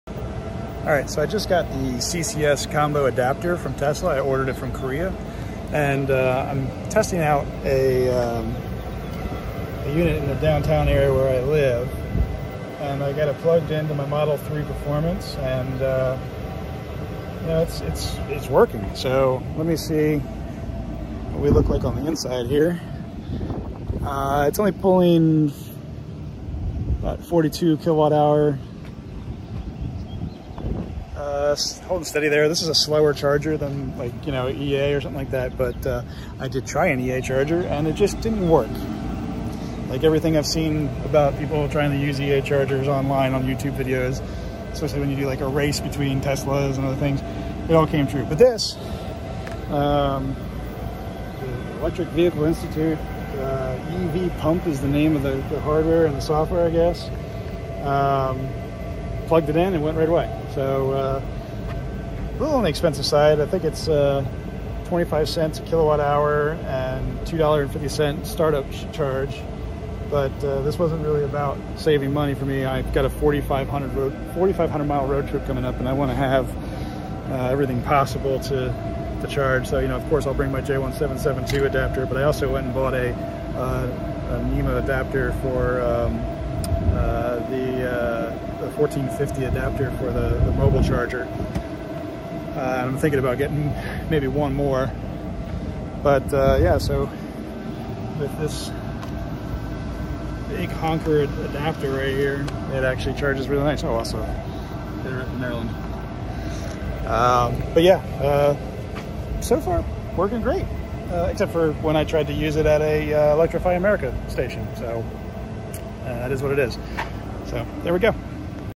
Testing the Tesla CCS Adapter for the first time with my ‘21 Model 3 Performance
Electrify America’s units at the Walmart near me were all unserviceable so I went to the next closest ccs unit which is SLOW. But the adapter worked immediately. With a 4,500ish mile-long road-trip coming up, wanted to have all options I can have available to me for charging.